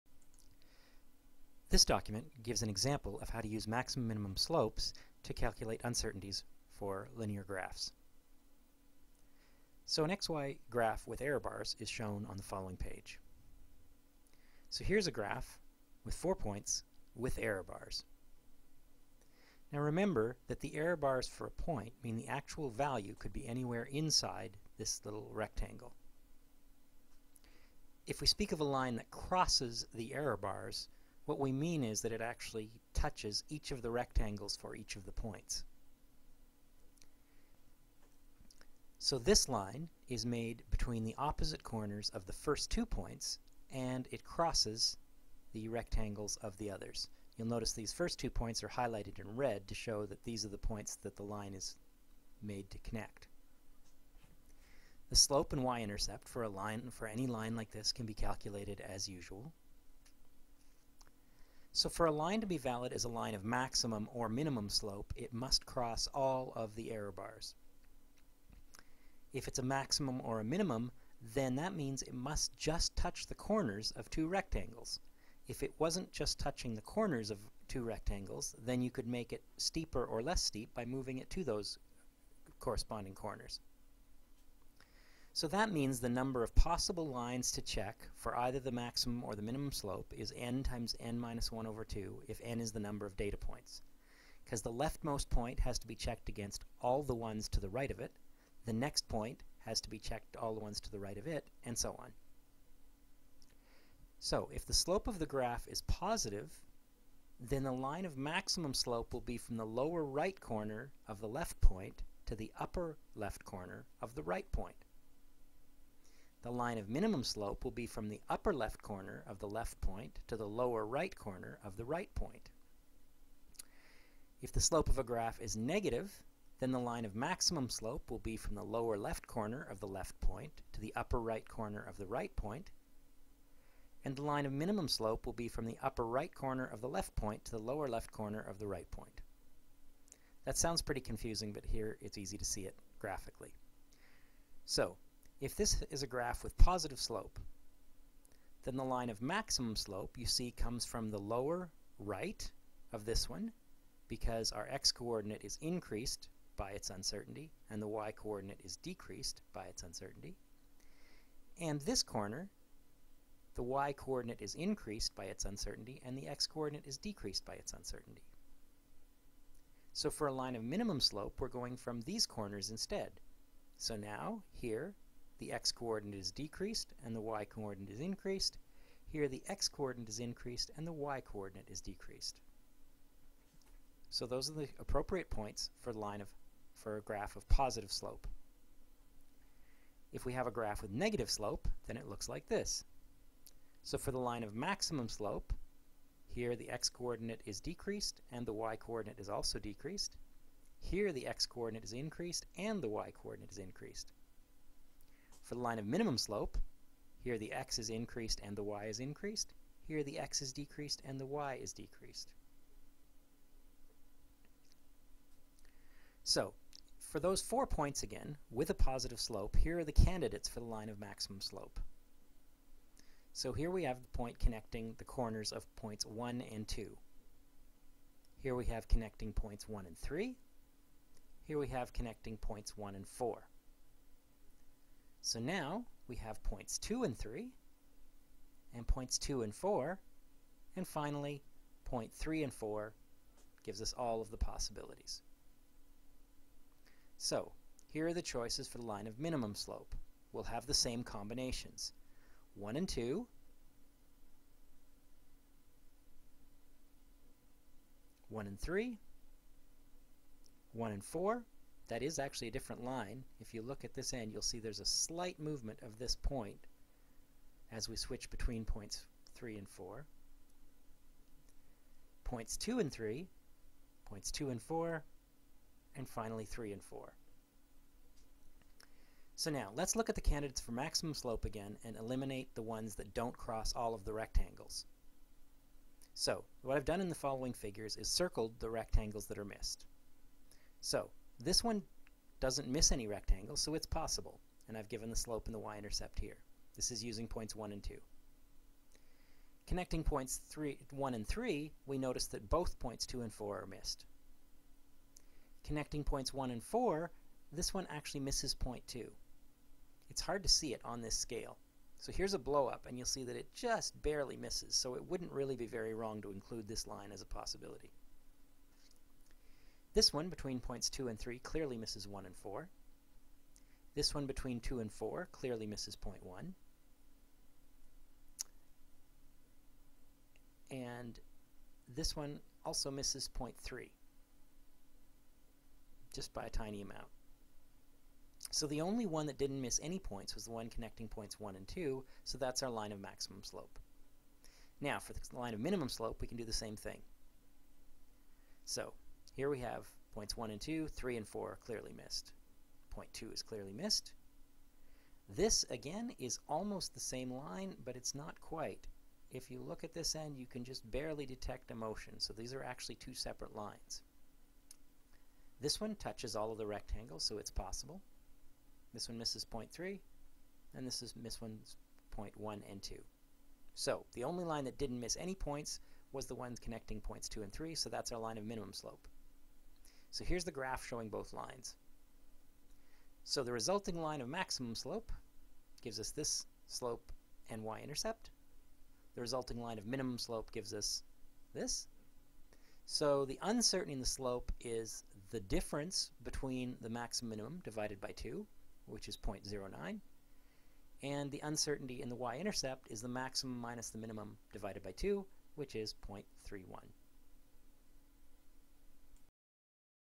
Example of Maximum and Minimum Slopes
How to choose which points to use for maximum and minimum slopes on linear graphs to determine uncertainties is the slope and y-intercept.

It's part of this playlist which has more on graphing, least squares fitting, etc.

WLU PC132